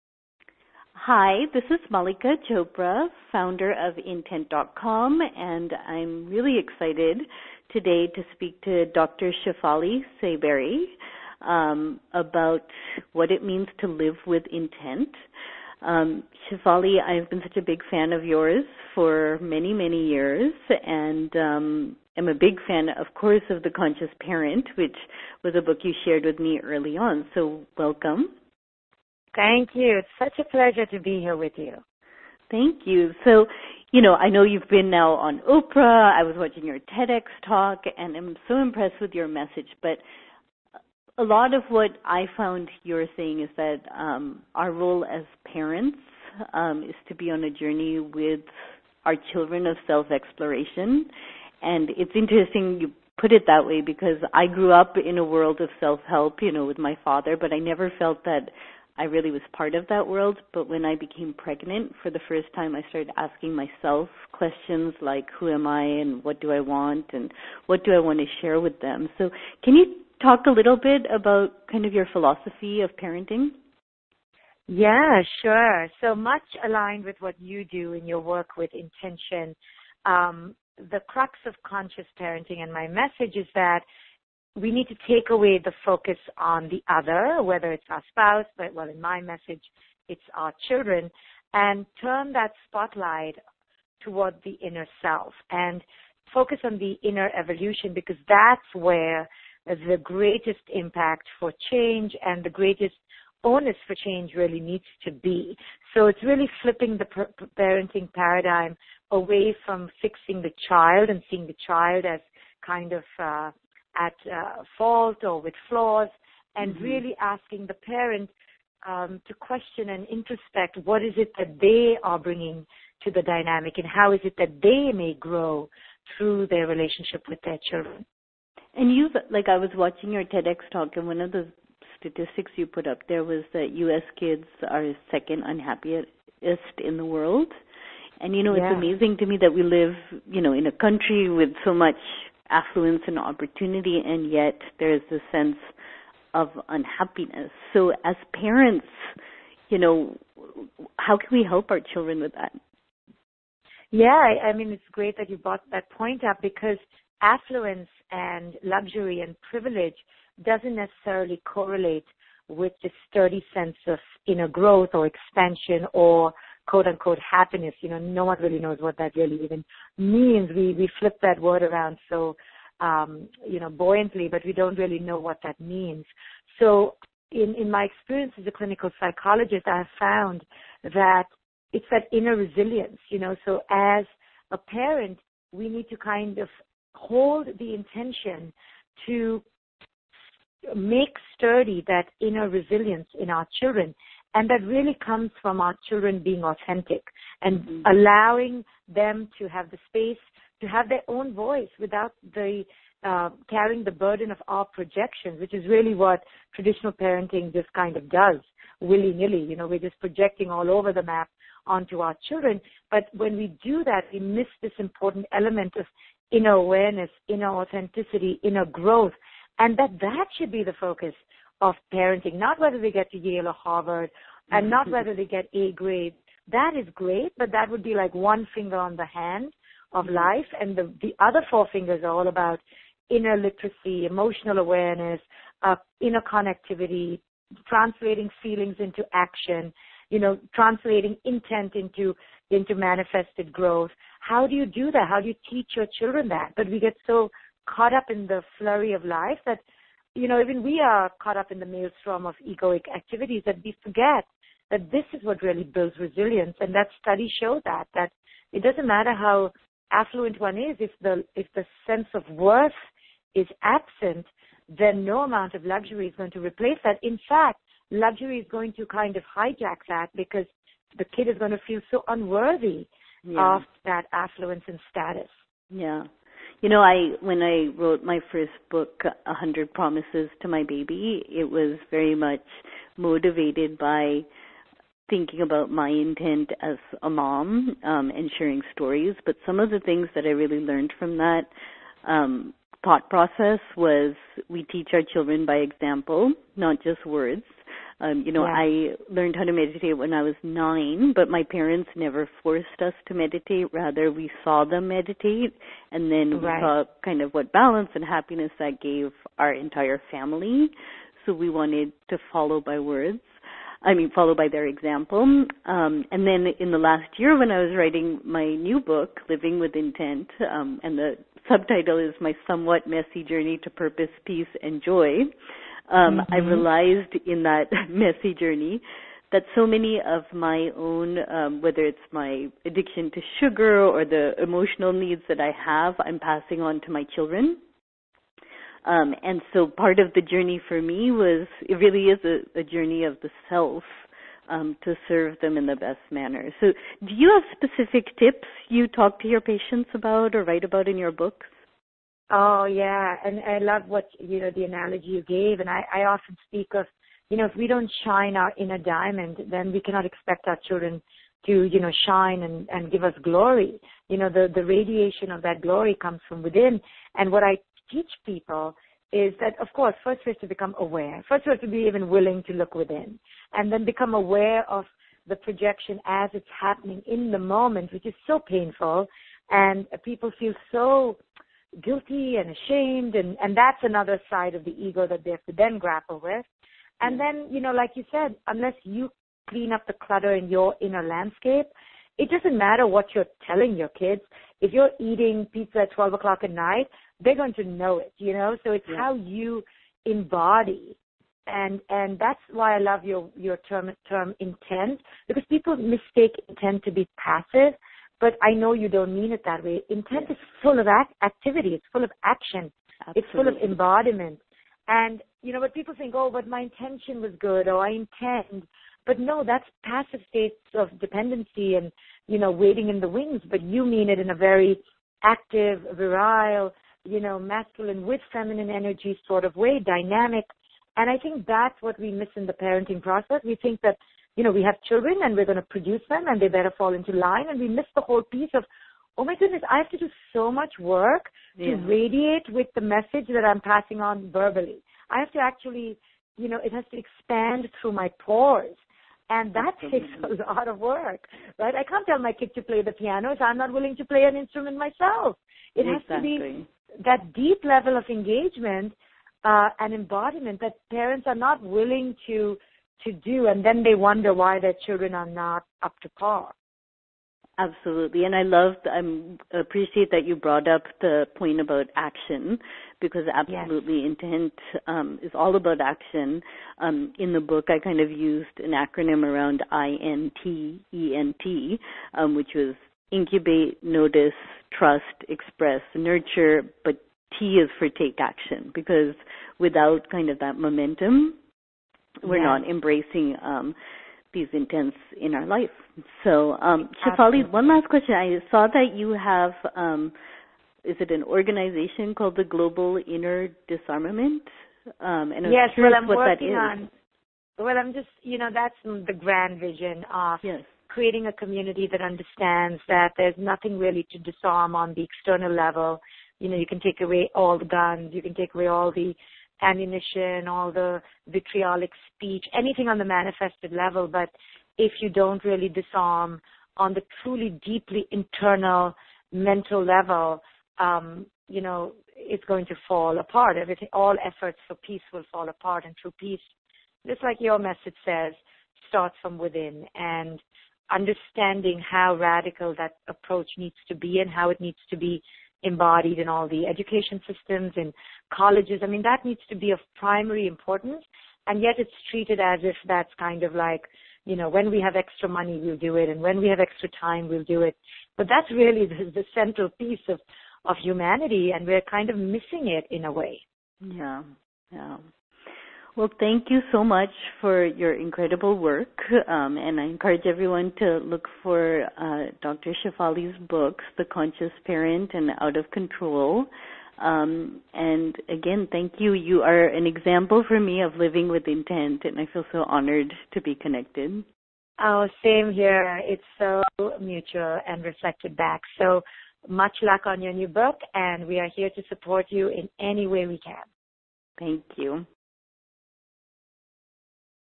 Livinig with Intent - Interview with Mallika Chopra and Dr Shefali Tsabary
Learn more and get your copy of Mallika's new book Living with Intent

For more information about Dr. Shefali Tsabary and her book, The Conscious Parent